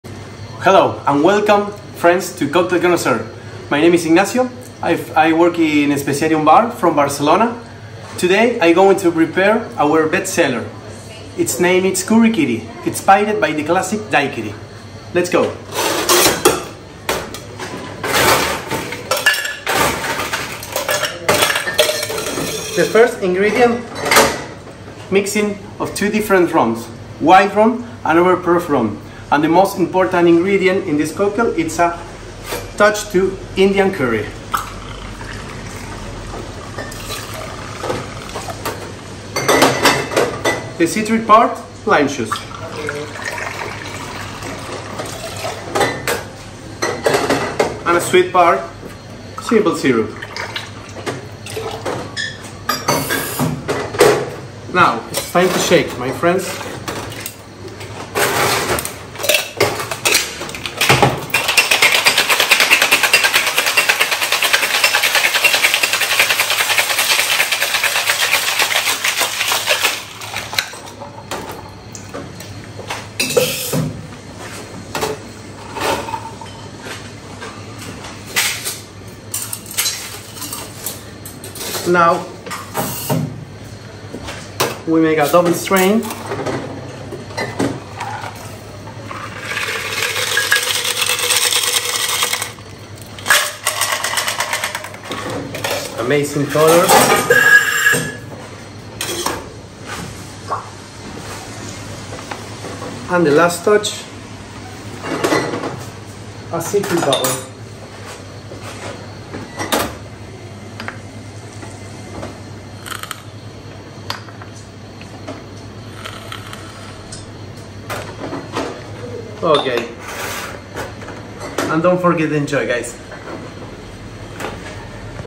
Ignacio Ceretti from Especiarium Bar (Barcelona): Curry Quiri
Ignacio Ceretti from Especiarium Bar makes us his twist on a classic cocktail, Curry Quiri